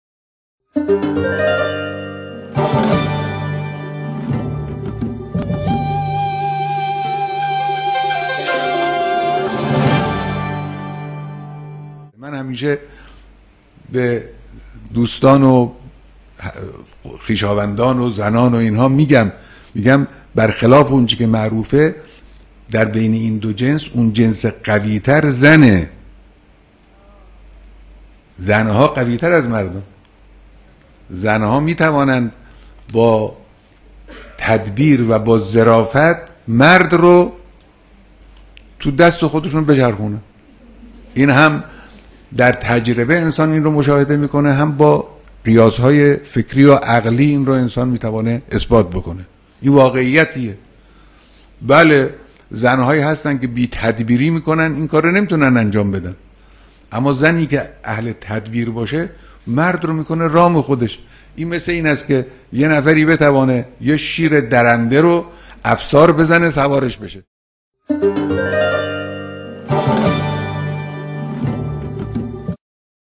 Imam Khamenei- Frauen sind stärker
Kurz vor Beginn des gesegneten Monats Radschab fanden sich graduierte und an den Universitäten und Theologischen Hochschulen, in Frauen- und Familienfragen, der Exekutive und den Medien und Nichtregierungsorganisationen sowie  Korankreisen tätige Frauen am Vormittag des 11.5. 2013 für zweieinhalb Stunden beim geehrten Revolutionsoberhaupt ein.

khamenei.ir